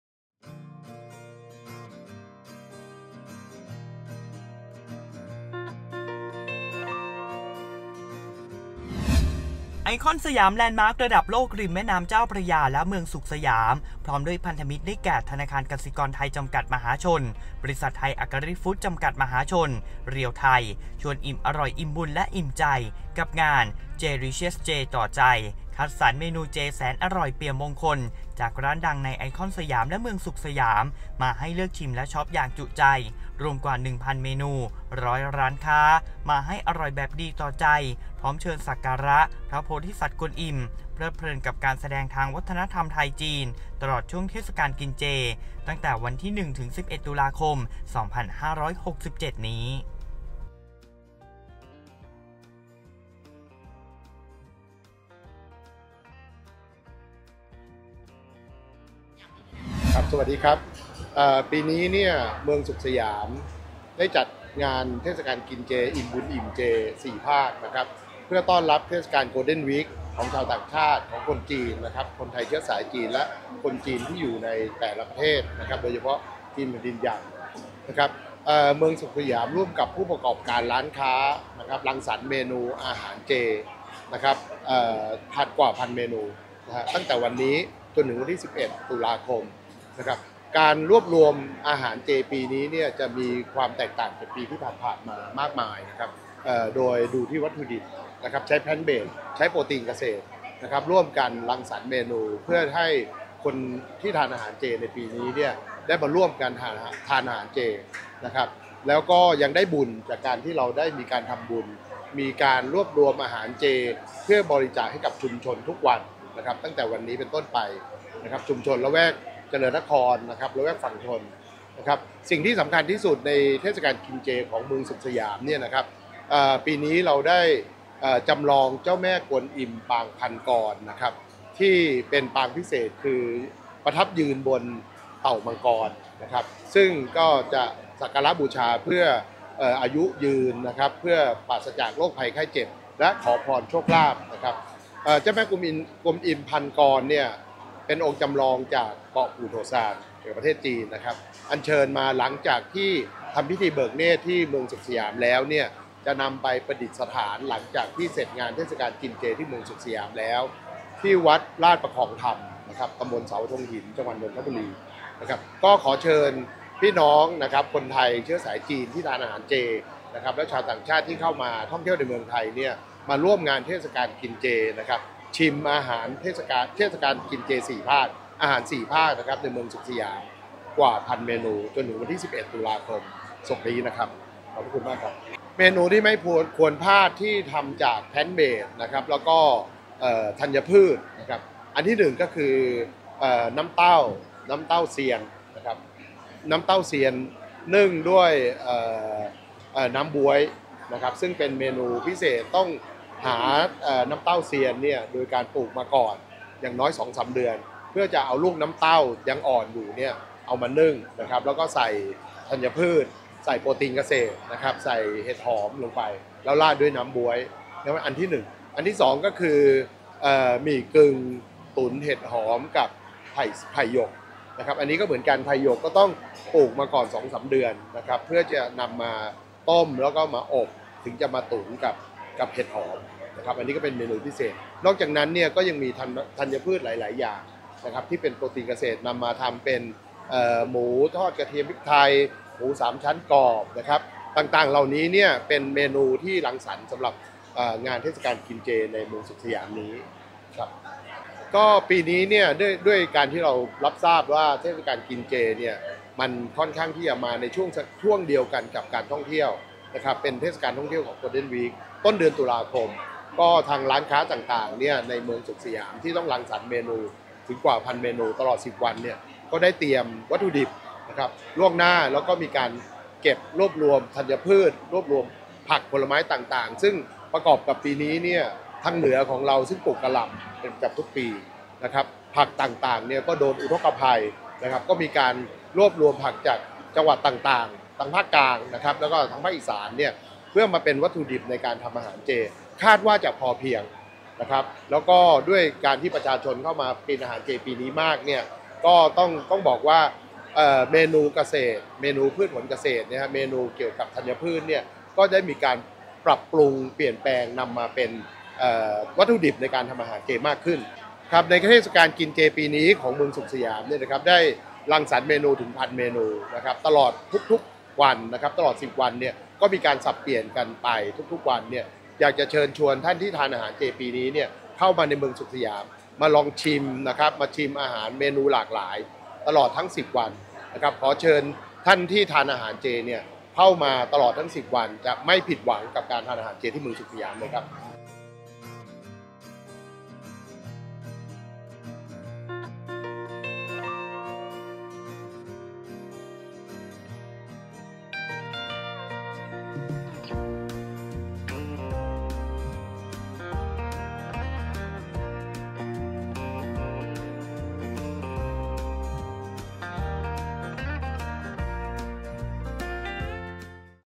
ไอคอนสยาม ต้อนรับเทศกาลเจสุดยิ่งใหญ่จัดงาน “J-licious เจต่อใจ”
ไอคอนสยาม ต้อนรับเทศกาลเจสุดยิ่งใหญ่
จัดงาน “J-licious เจต่อใจ” ยกทัพเมนูเจจากร้านดังมาให้อิ่มบุญ อิ่มใจ
ตลอดเทศกาลกินเจ 1-11 ตุลาคม 2567

  ไอคอนสยาม แลนด์มาร์กระดับโลกริมแม่น้ำเจ้าพระยา และเมืองสุขสยาม พร้อมด้วยพันธมิตร ได้แก่ ธนาคารกสิกรไทย จำกัด (มหาชน) , บริษัท ไทย อกริ ฟู้ดส์ จำกัด (มหาชน) (เรียลไทย) ชวนอิ่มอร่อย อิ่มบุญ และอิ่มใจ กับงาน “J-licious เจต่อใจ” คัดสรรเมนูเจแสนอร่อย เปี่ยมมงคล จากร้านดังในไอคอนสยาม และเมืองสุขสยาม   มาให้เลือกชิมและช้อปอย่างจุใจ รวมกว่า 1,000 เมนู 100 ร้านค้า มาให้อร่อยแบบดีต่อใจ พร้อมเชิญสักการะพระโพธิสัตว์กวนอิม เพลิดเพลินกับการแสดงทางวัฒนธรรมไทย-จีน ตลอดช่วงเทศกาลกินเจ ตั้งแต่วันที่ 1-11 ตุลาคม 2567